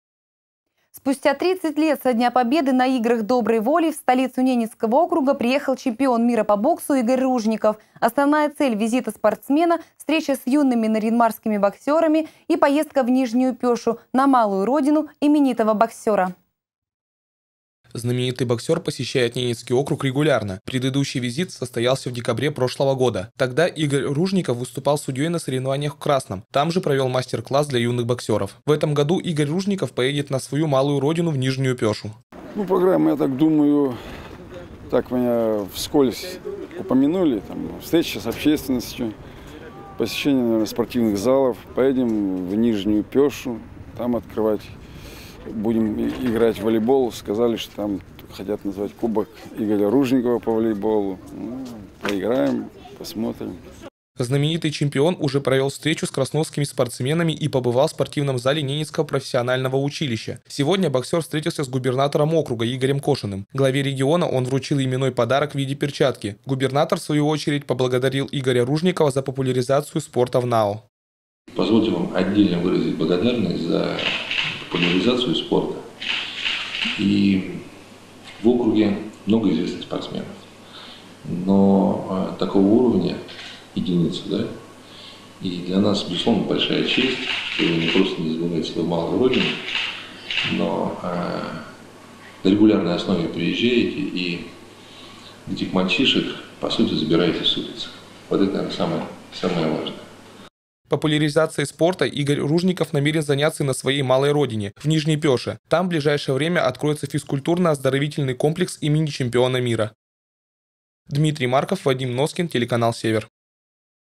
В НАО прибыл чемпион мира по боксу Игорь Ружников
В столицу Ненецкого округа приехал чемпион мира по боксу Игорь Ружников. Основная цель визита спортсмена — встреча с юными нарьян-марскими боксерами и поездка в Нижнюю Пёшу, на малую родину именитого боксера.

Спортсмен посещает Ненецкий округ регулярно. Предыдущий визит состоялся в декабре прошлого года. Тогда Игорь Ружников выступал судьей на соревнованиях в поселке Красное, там же провел мастер-классы для юных боксеров. В этом году Игорь Ружников отправится на свою малую родину — в Нижнюю Пёшу.